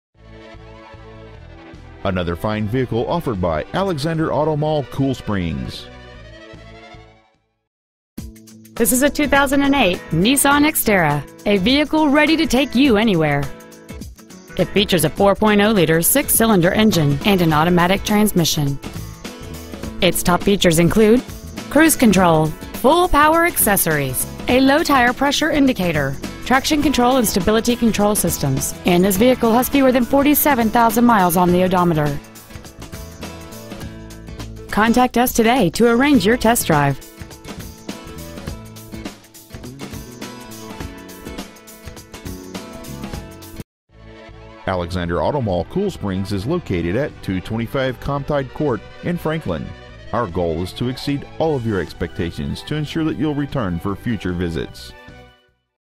Pre-Owned 2008 Nissan Xterra Franklin TN
Year: 2008
 Make: Nissan
 Model: Xterra
 Engine: 4.0L V6
 Trans.: AUTO W/OD
 Exterior: Dark Gray
 Miles: 46,918
 Interior: Steelgraphite

 2008 Nissan Xterra Franklin TN